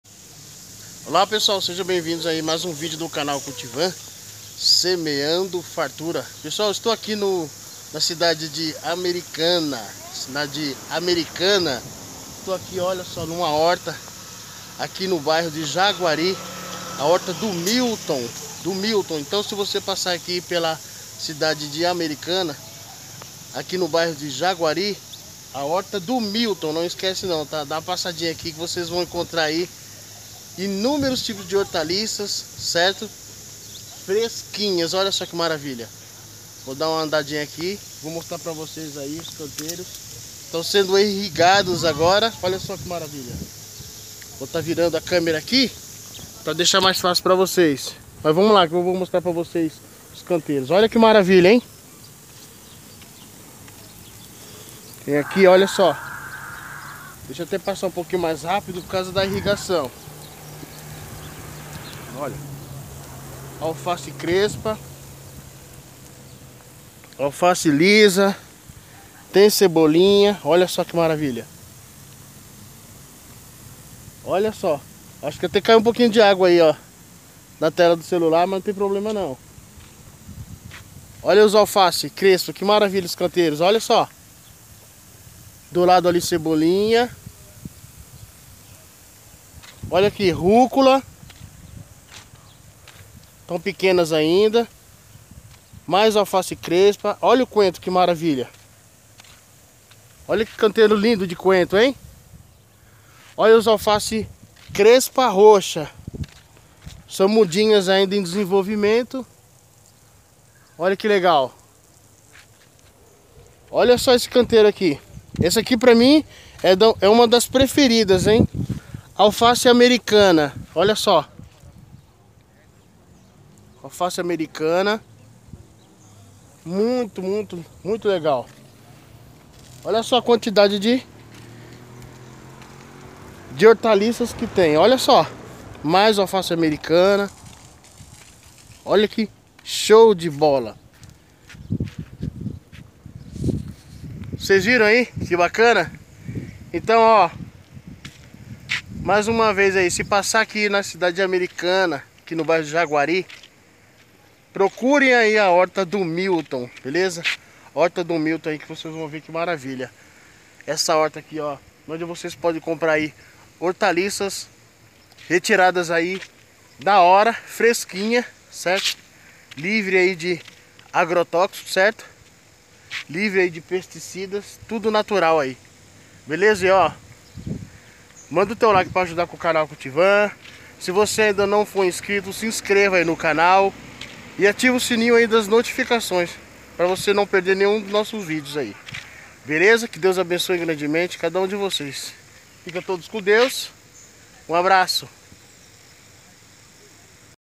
visitando uma linda horta em Americana.
Horta orgânica e sem agrotóxicos,Tudo fresquinho,Direto do canteiro para mesa de quem passa na cidade de,Americana no bairro de Jaguari na,Na horta do Milton vale a pena passar e levar uma bela sacola.

Deus abençoe todos vocês, abraço